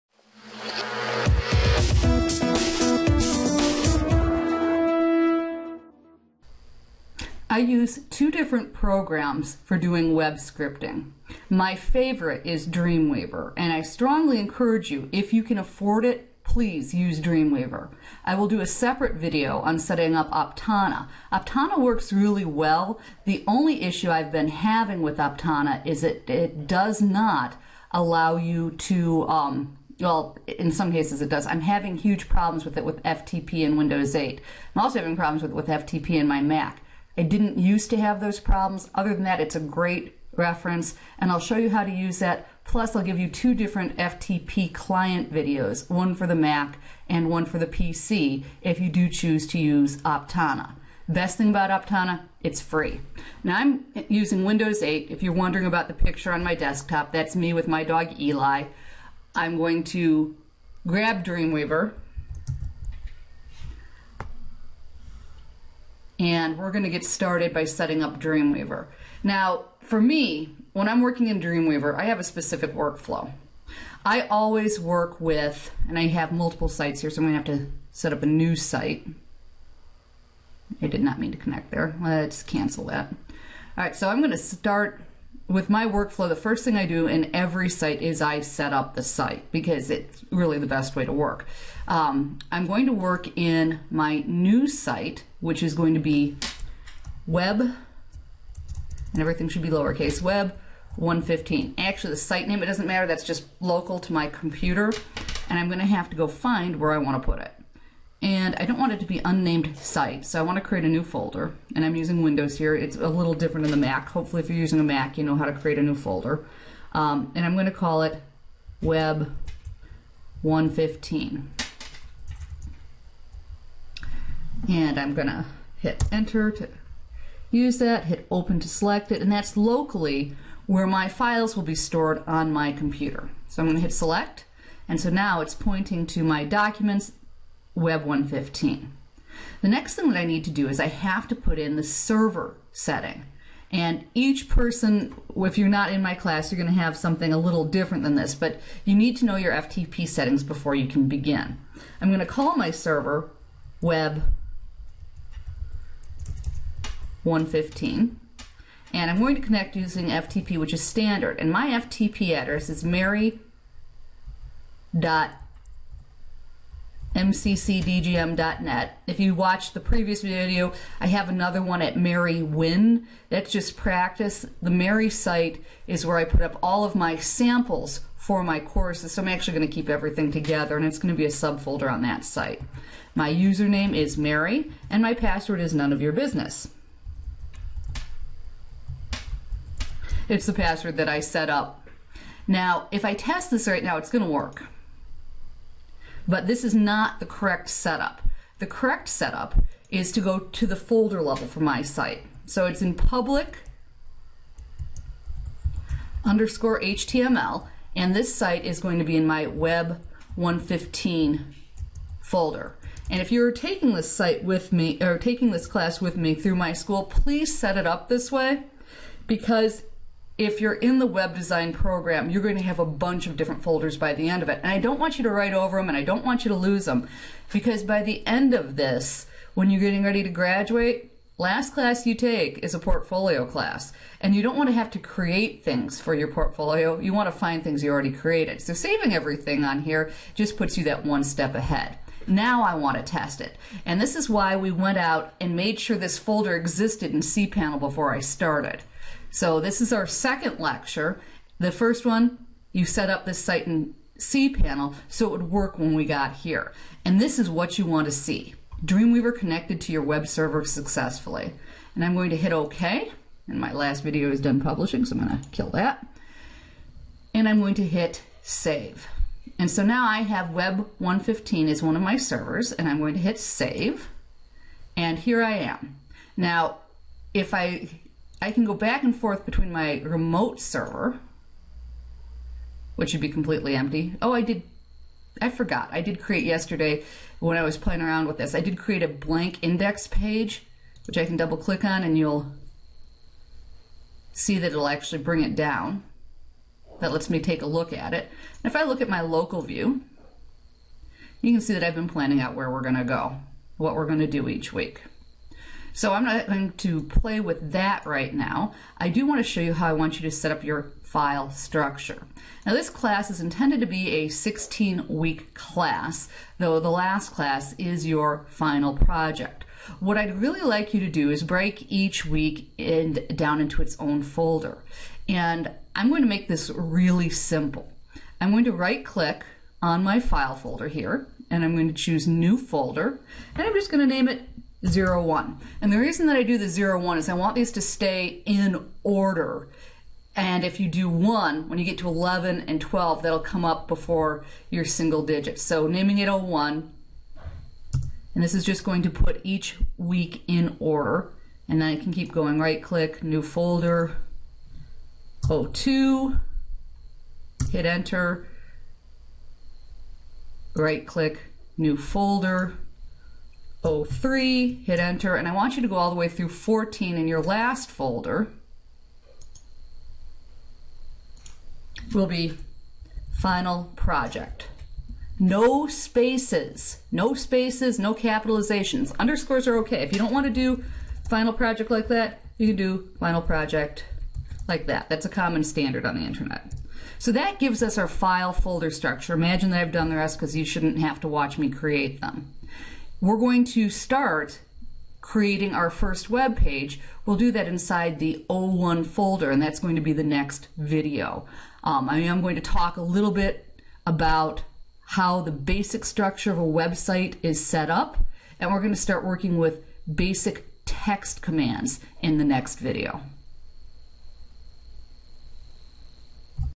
HTML & CSS - Lectue 2 - setting up dreamweaver for class
Setting up dreamweaver for the HTML & CSS course